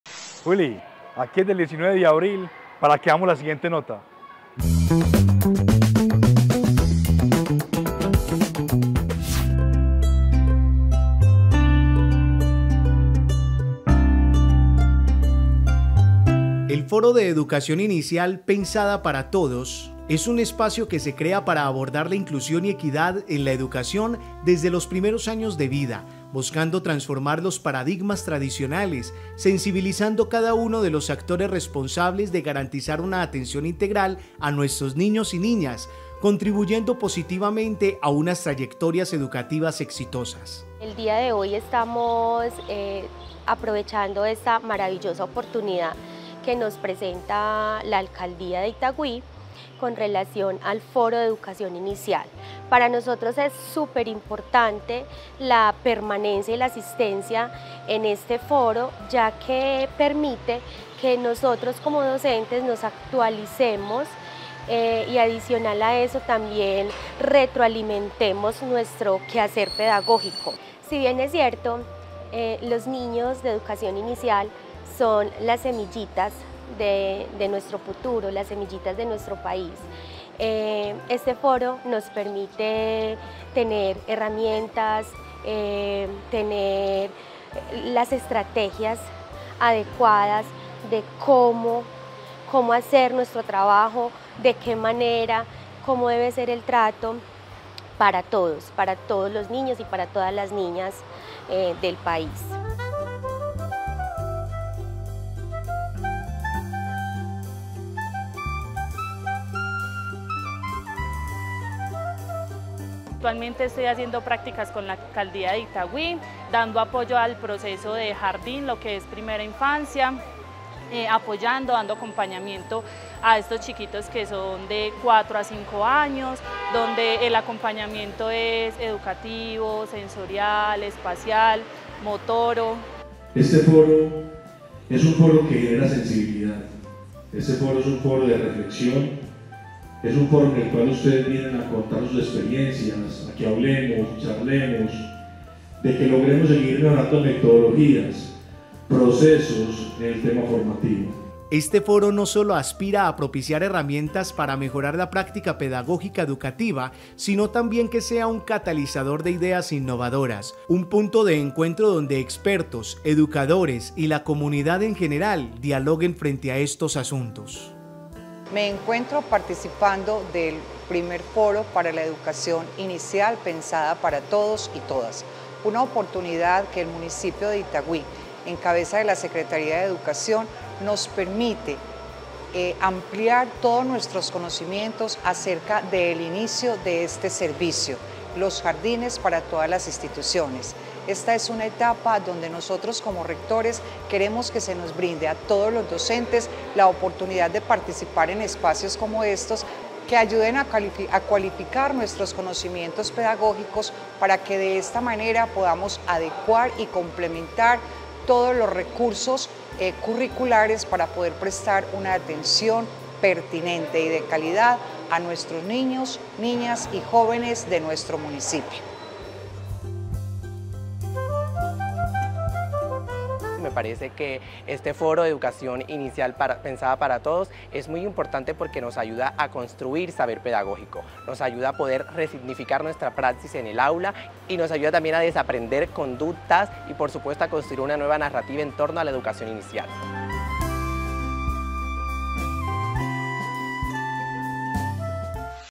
TODOS SOMOS ITAGÜÍ - Foro de educación inicial
Foro de educación inicial, espacio pensado para abordar la inclusión y equidad en la educación, desde los primeros años de vida